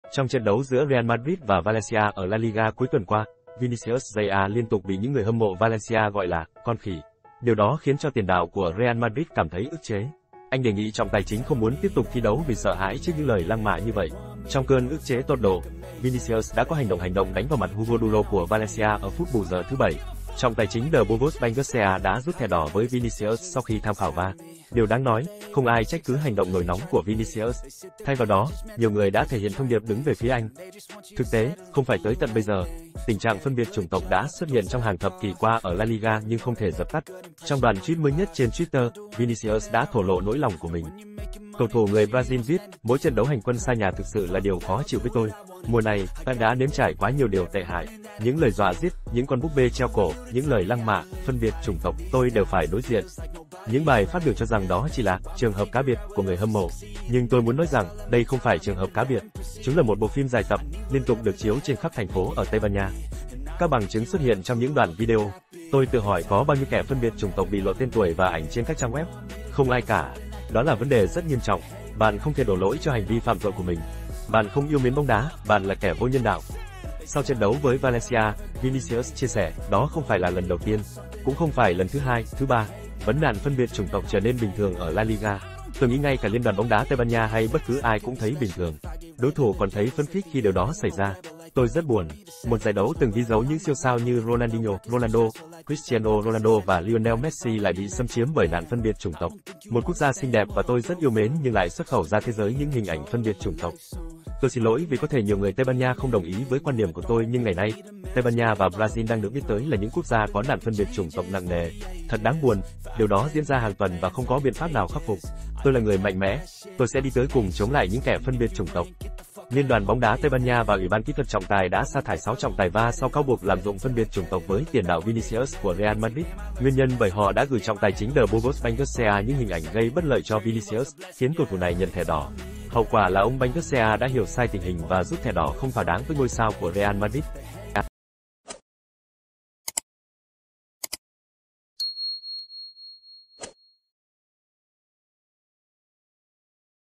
SAO TRẺ VINICIUS CỦA REAL MADRID BỊ GIỌA GIẾT VÀ LĂNG MẠ, BÓNG ĐÁ TÂY BAN NHA HỖN LOẠN
Trong trận đấu giữa Real Madrid và Valencia ở La Liga cuối tuần qua, Vinicius Jr liên tục bị những người hâm mộ Valencia gọi là "con khỉ". Điều đó khiến cho tiền đạo của Real Madrid cảm thấy ức chế. Anh đề nghị trọng tài chính không muốn tiếp tục thi đấu vì sợ hãi trước những lời lăng mạ như vậy. Trong cơn ức chế tột độ, Vinicius  đã có hành động hành động đánh vào mặt Hugo Duro của Valencia ở phút bù giờ thứ 7. Trọng tài chính De Burgos Bengoetxea đã rút thẻ đỏ với Vinicius sau khi tham khảo VAR.  Điều đáng nói, không ai trách cứ hành động nổi nóng của Vinicius. Thay vào đó, nhiều người đã thể hiện thông điệp đứng về phía anh.